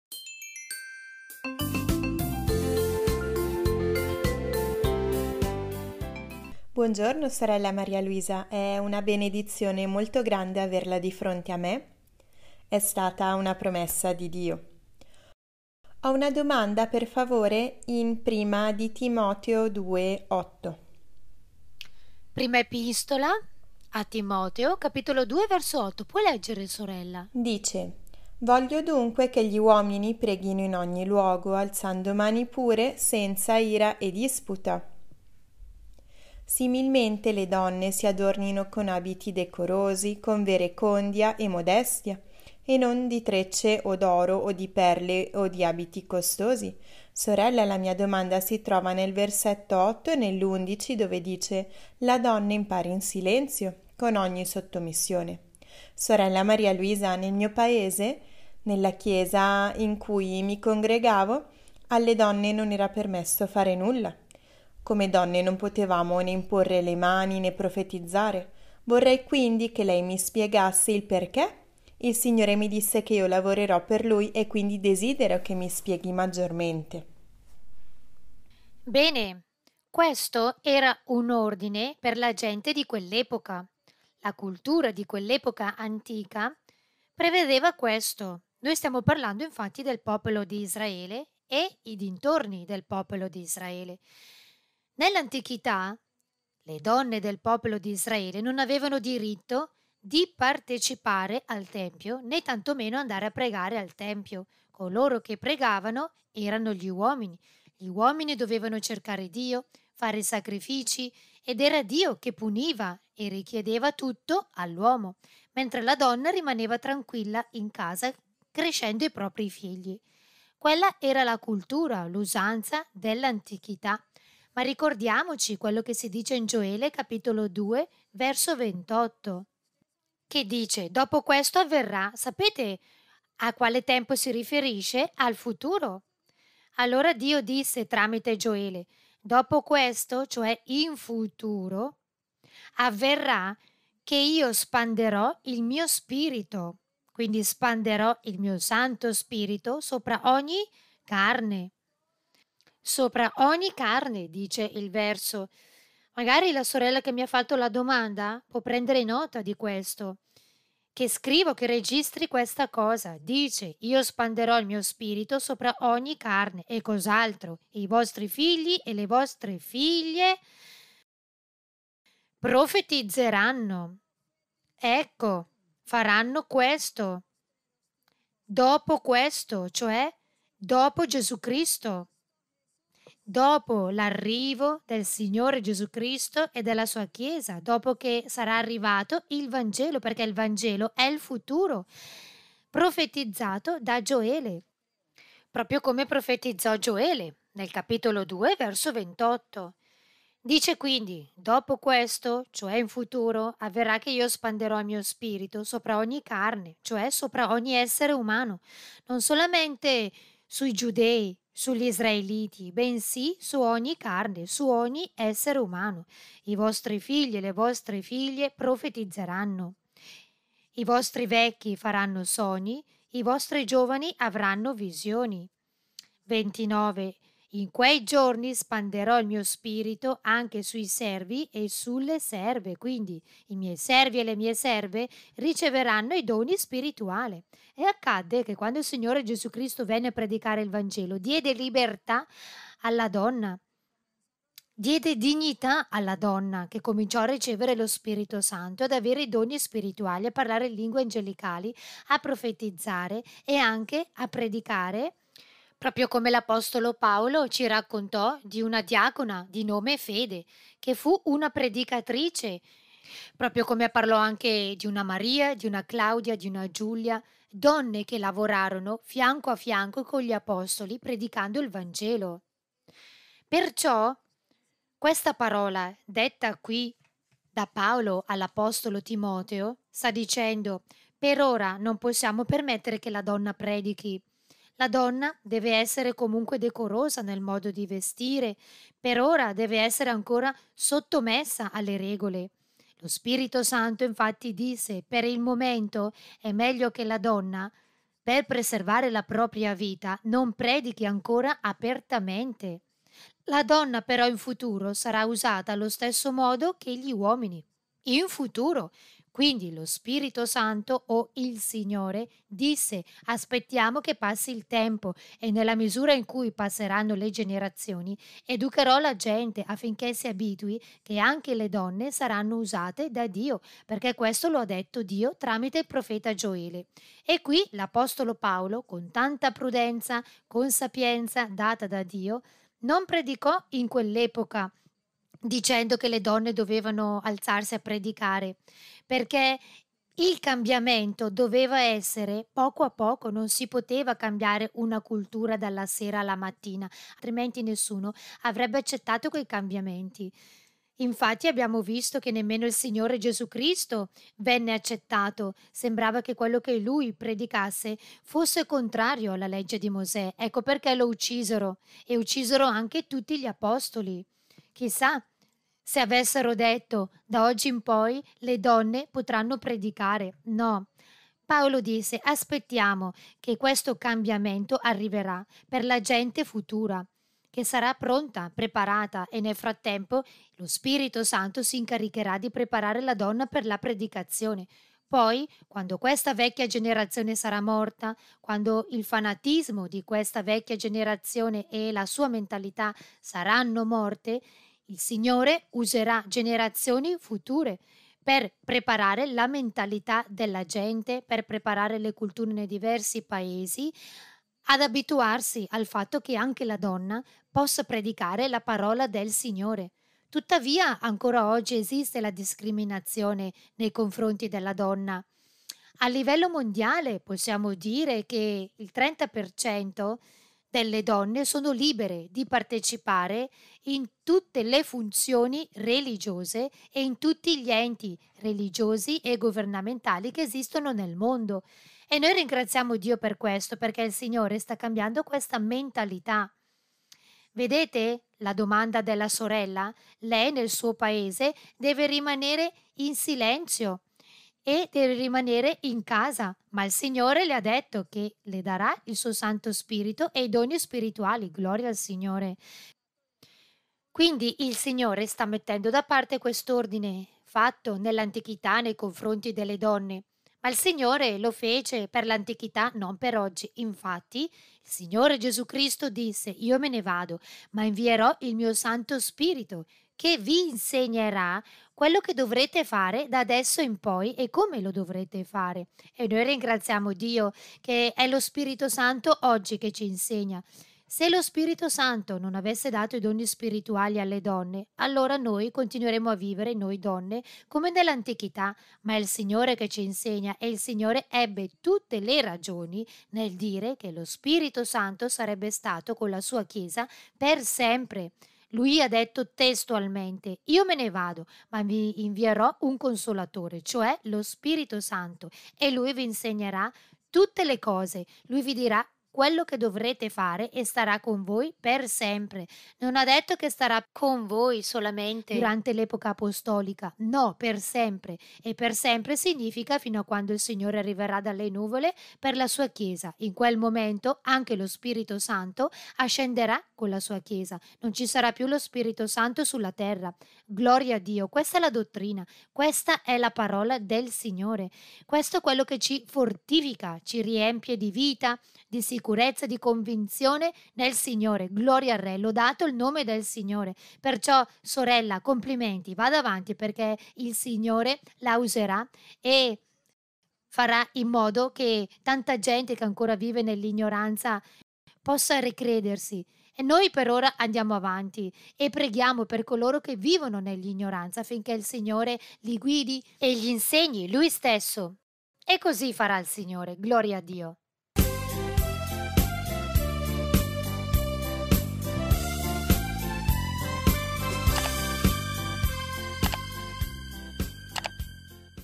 Il lavoro spirituale della donna in Chiesa, 1^ Timoteo 2 : 8 - 11, Sorella Maria Luisa Piraquive
Chiesa di Dio Ministeriale di Gesù Cristo Internazionale.